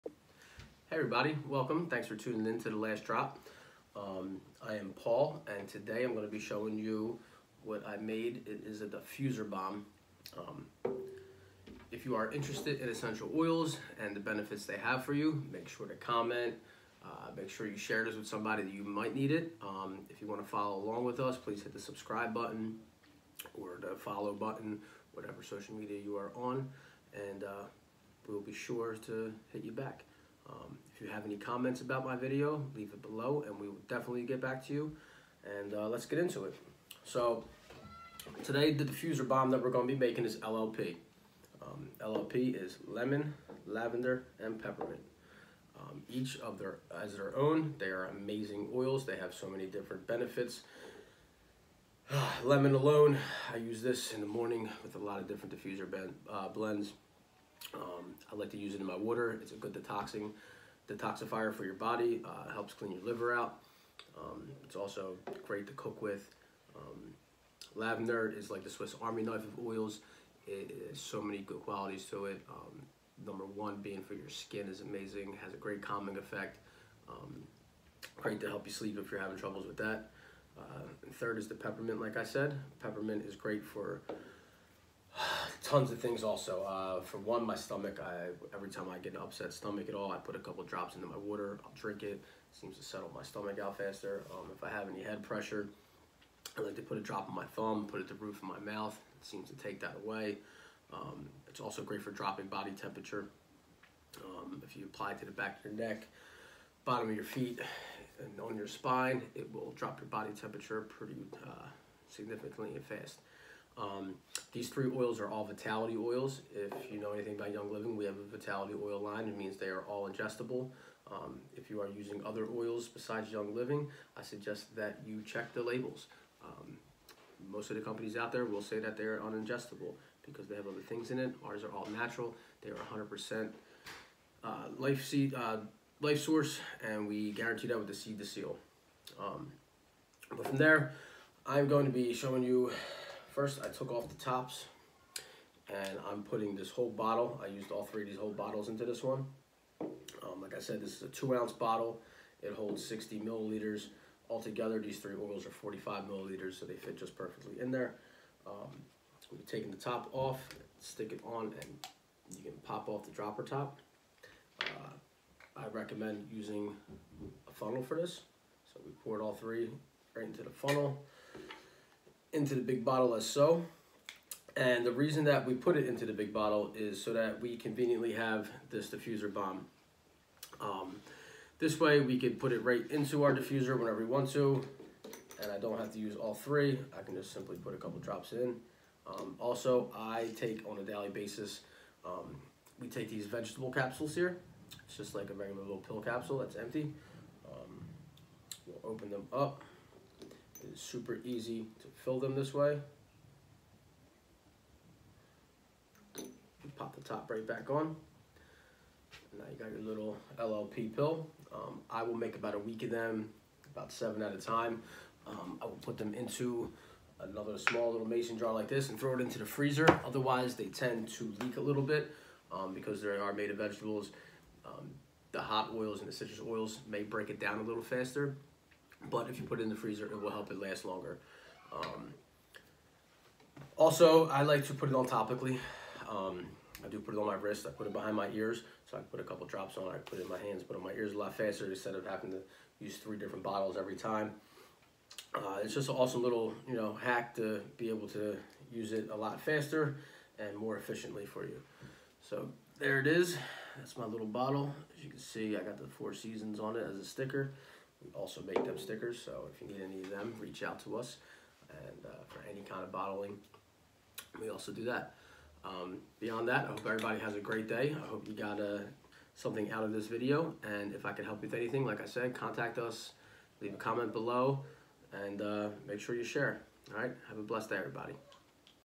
Young Living Essential Oils DIY Diffuser Bomb
Here I will be sharing my DIY OIL HACK for dealing with seasonal nuisances. If you suffer from dry or tearing eyes, scratching throat, or running noises this blend bomb is for you!